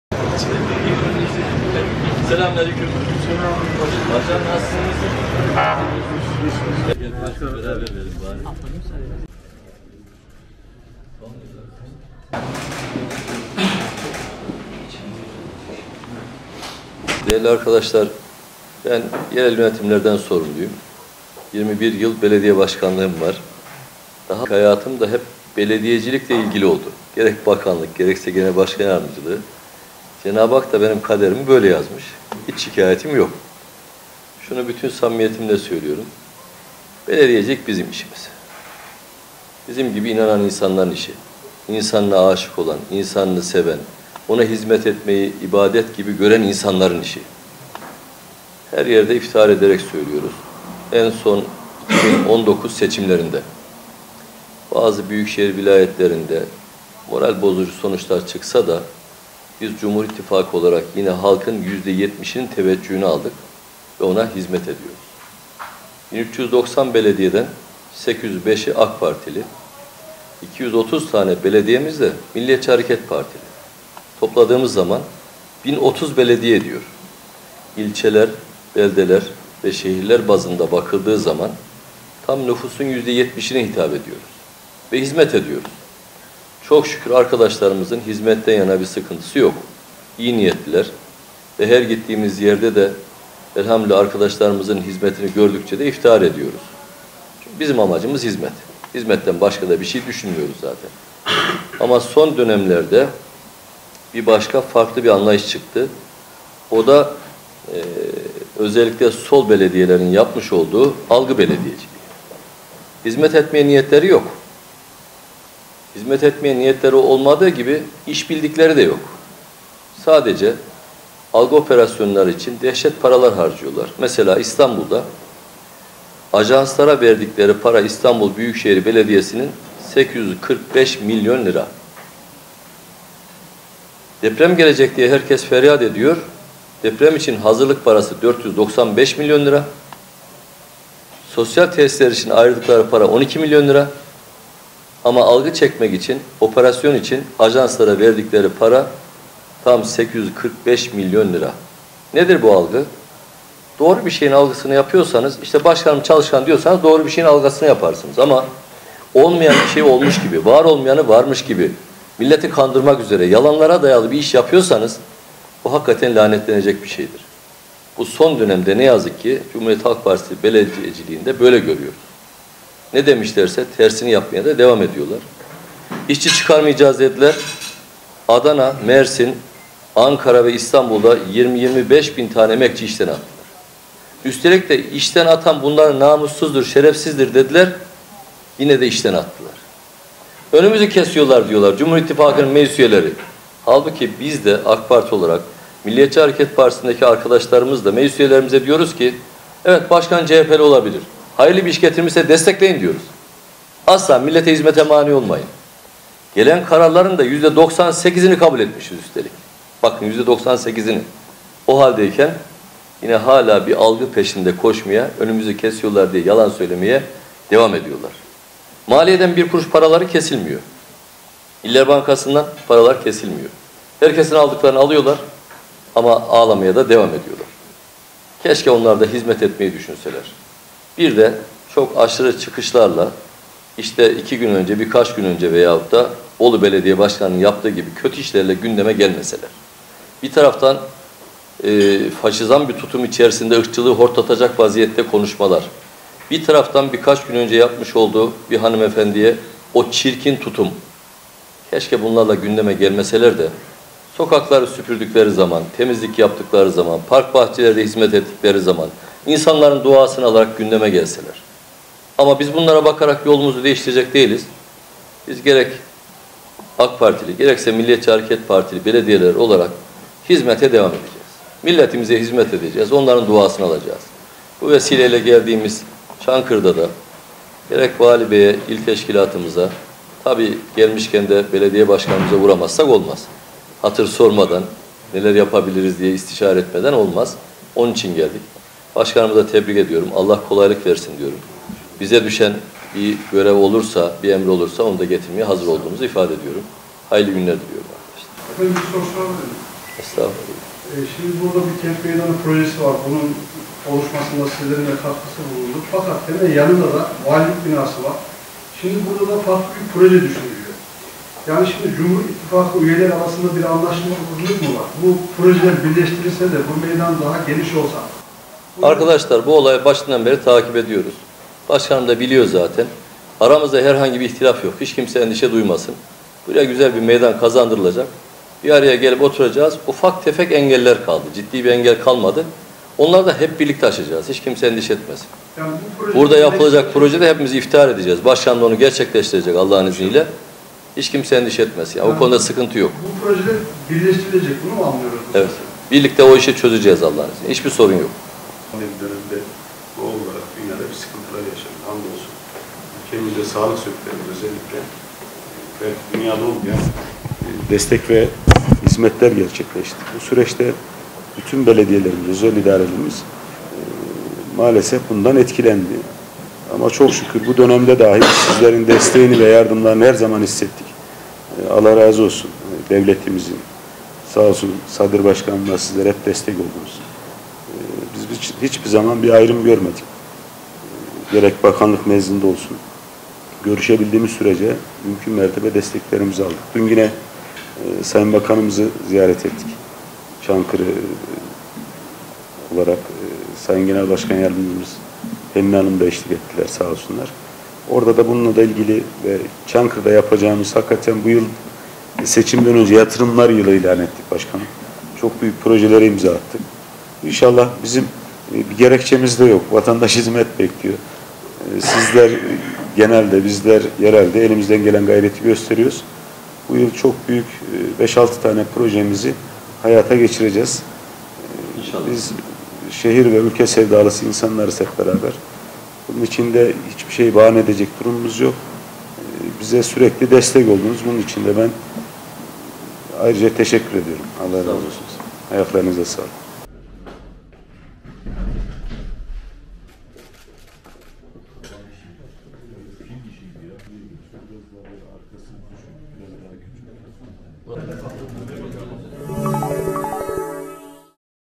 Mehmet Özhaseki’den kent meydanı açıklaması!
AK Parti Yerel Yönetimler Başkanı ve Genel Başkan Yardımcısı Mehmet Özhaseki, Çankırı Belediyesi ziyaretinde kent meydanı ile ilgili açıklama yaptı.
Çankırı Postası Genel Yayın Yönetmenimiz Ercan Şeker, kent meydanı ve valilik alanın da iki ayrı proje olduğuna dikkat çekti. Cumhur ittifakı arasında anlaşmazlık mı var? Projeler birleştirilerek meydan neden büyütülmüyor” diye soran Şeker’e Mehmet Özhaseki’den yanıt geldi.